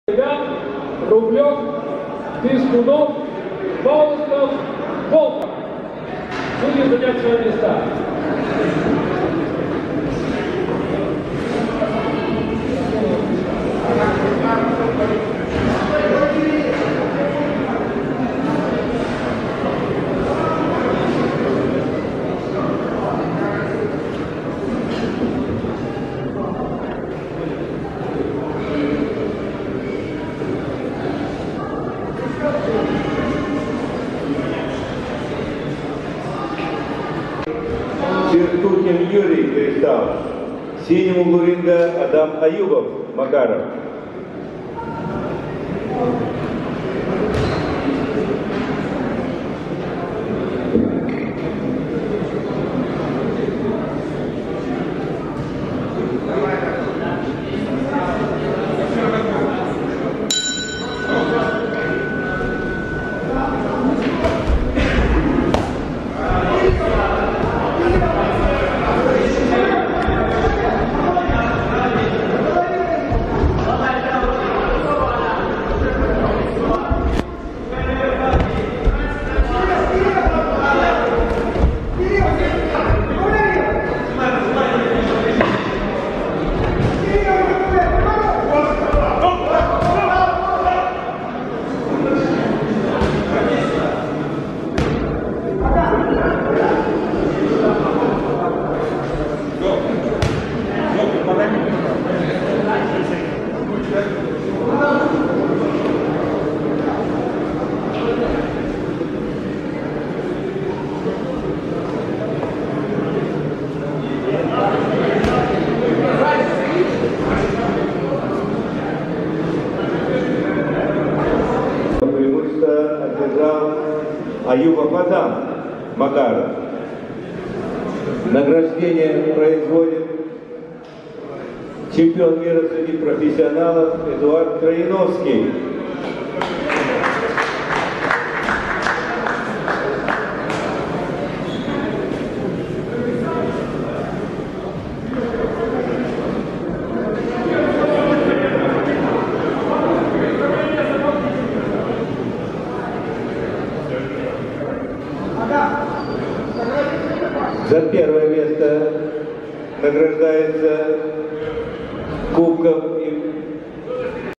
Чертухин Юрий(Кристалл)+Аюбов Адам(Макаров)86кг.
Юность Сахалина - Бокс-(Финал) - 13 ноября 2021